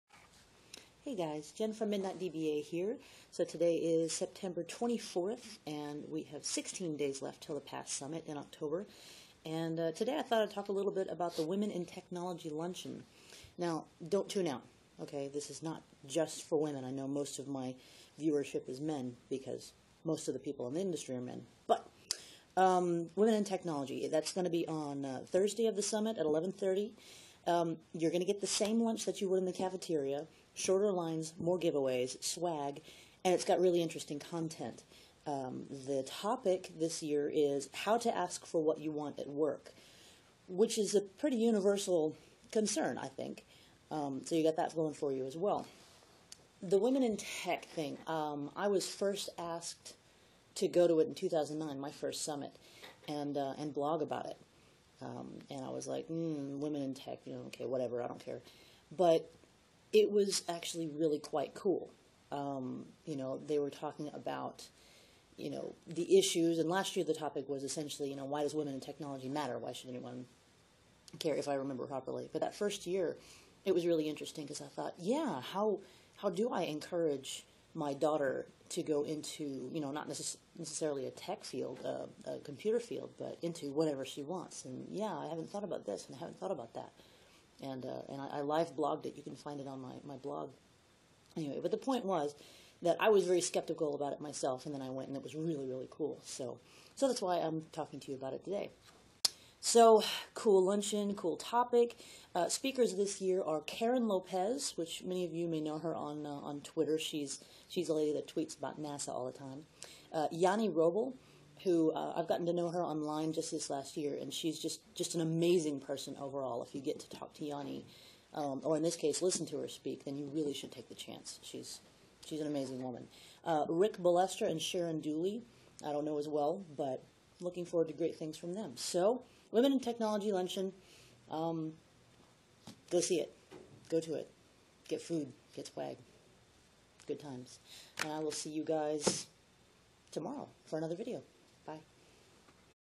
PASS Summit 2011: T-16 Days (WIT Luncheon)
I really-really REEEEALLY like the PASS Summit in Seattle. There's a lot going on with us and with PASS, so I'll post a few of these little vids in prep for the big event.

In this 2.5 minute episode: The Women in Technology Luncheon at the PASS Summit 2011, and why you (yes, YOU Sir-or-Madam!) should attend!

Here is a blog of mine that gives a good description of all things WITty in PASS

And here is my SQL University WIT blog "Teach Your Children Well"